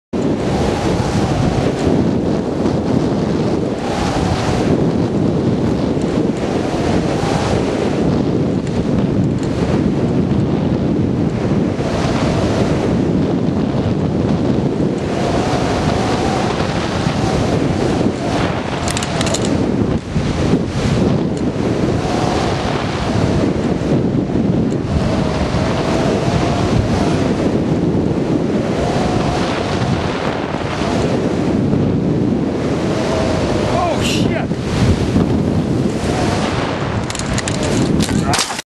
hang glider crash Rampart
dont chagge you plan at last moment